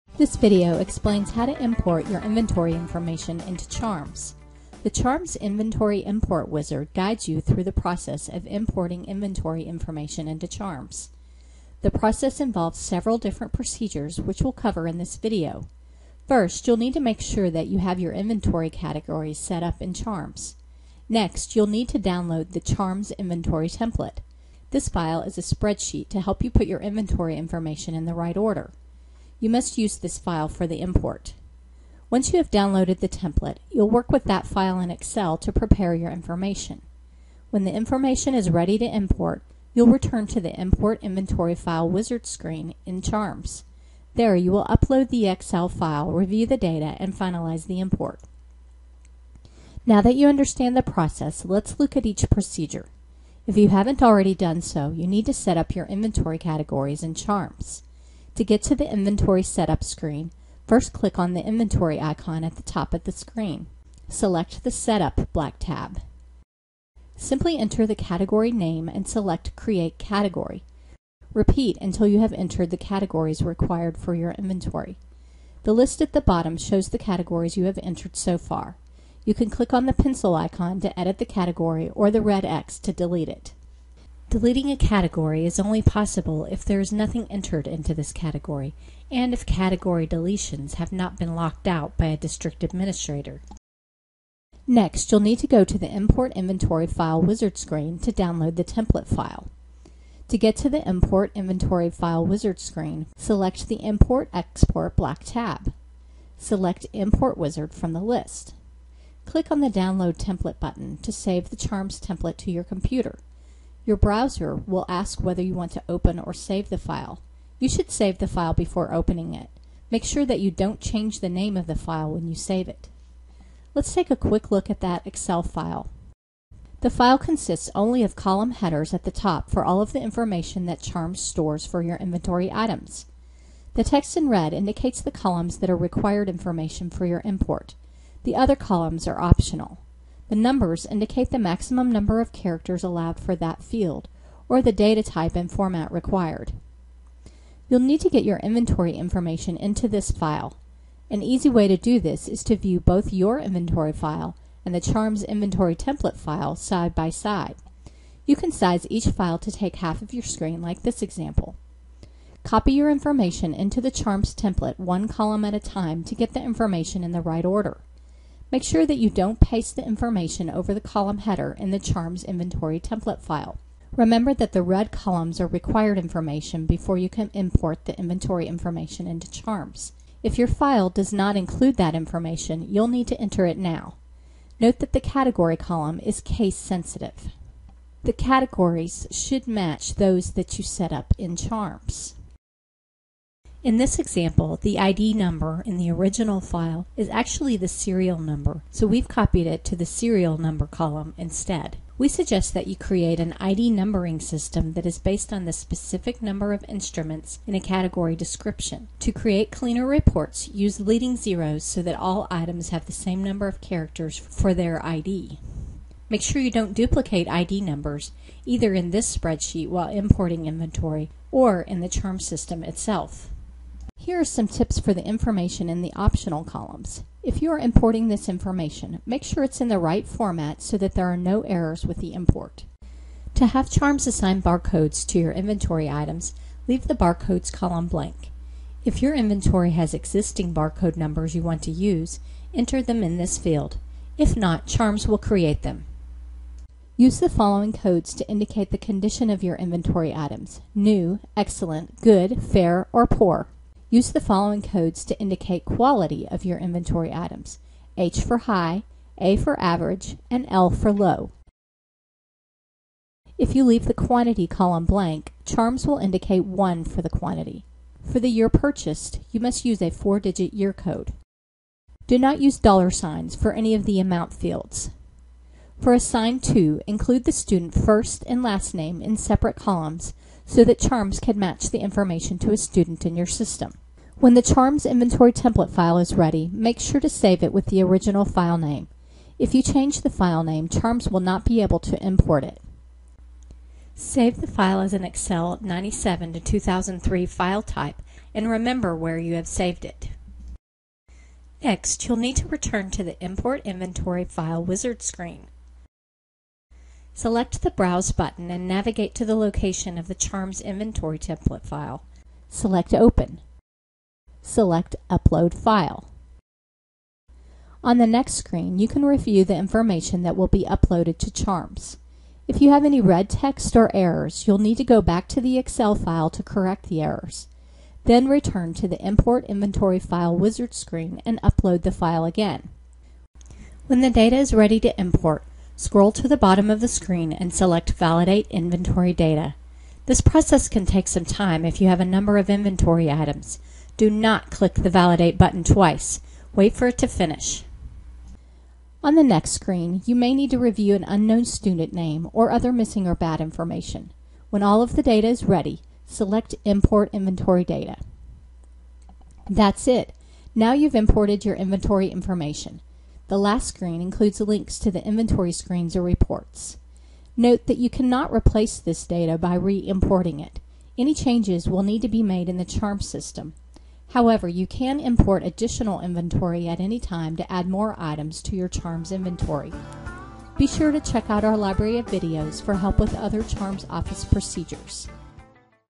Inventory Import Wizard
Working with the excel import template to upload your Inventory